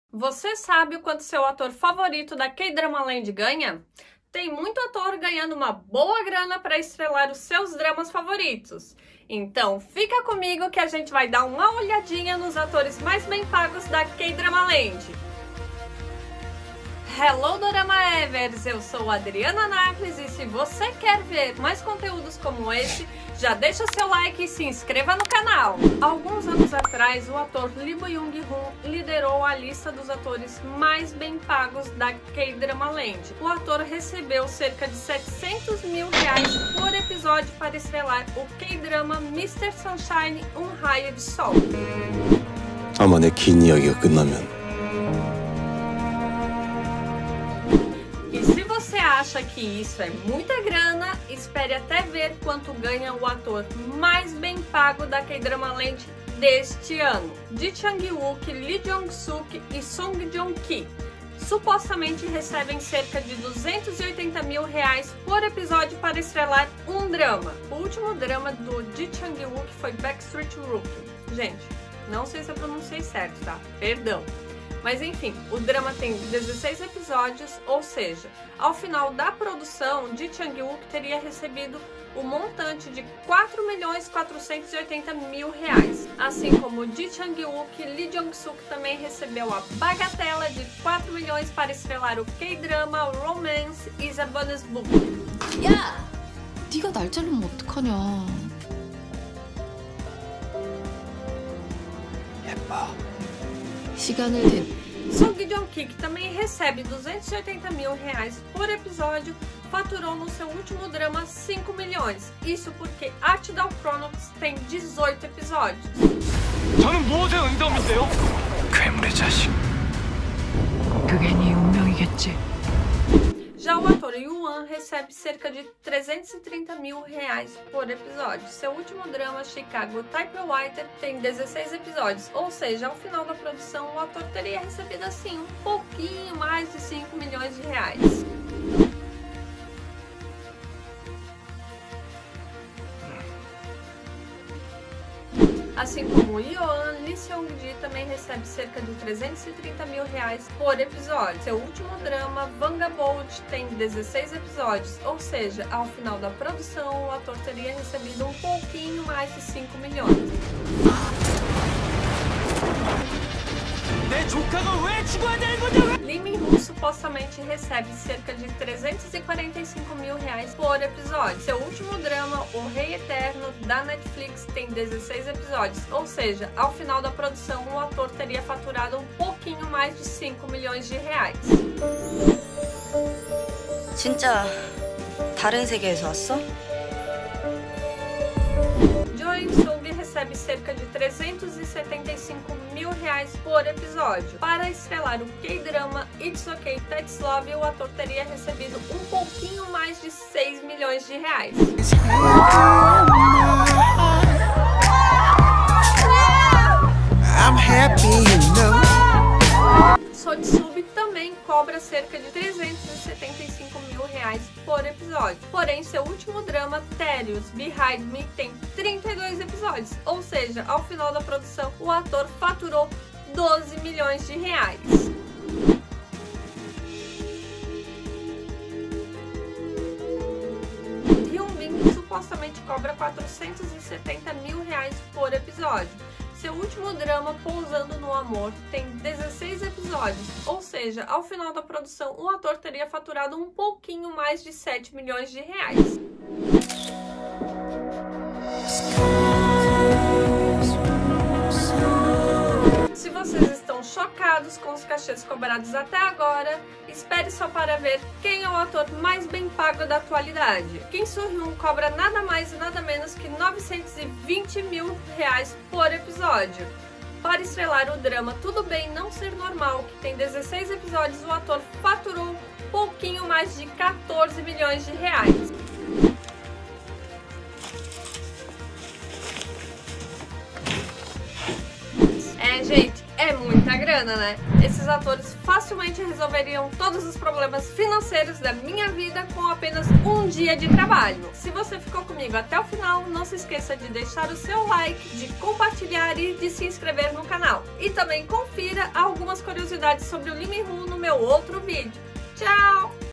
Salarios dos atores coreanos - Os atores mais bem pagos da k-dramaland
Descubra quem são os astros e estrelas que brilham não só nas telas, mas também nos cheques de pagamento em K-dramaland! Neste vídeo, mergulhamos no mundo glamouroso dos dramas coreanos para revelar os salários dos atores mais bem pagos. Você ficará surpreso ao saber quem lidera a lista e quanto os principais atores de K-dramas realmente ganham por episódio. Desde os veteranos até as estrelas em ascensão, conheça os ícones que estão conquistando corações e enchendo os bolsos com seus papéis cativantes. Não perca essa análise exclusiva dos salários que definem o padrão de ouro em K-dramaland!

Assista e veja os salários dos atores coreanos e possivelmente os atores coreanos mais ricos.